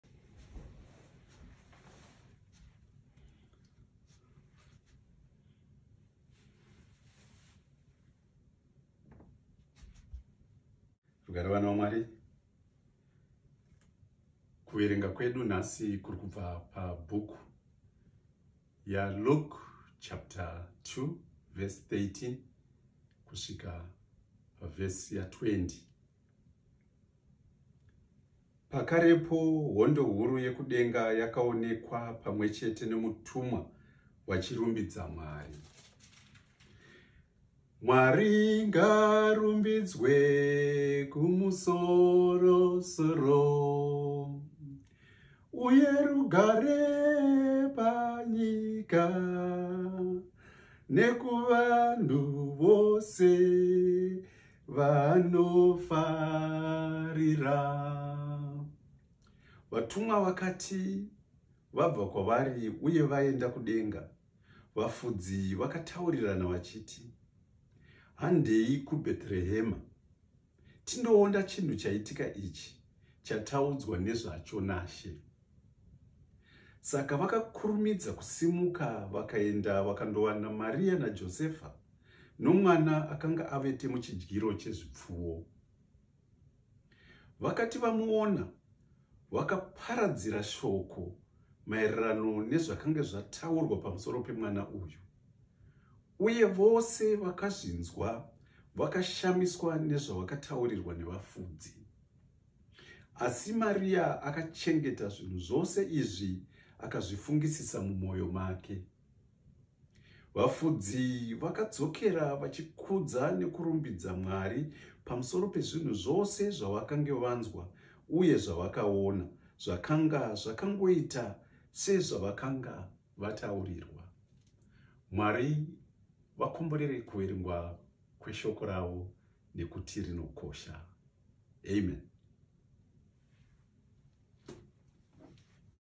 Jesus Christ is Born
Luke 2:13-20 to the lost tribes of Israel a freedom fighter was send but they denied Him but to the Gentiles a Saviour died for us and we accepted it. In God we trust always. Amen.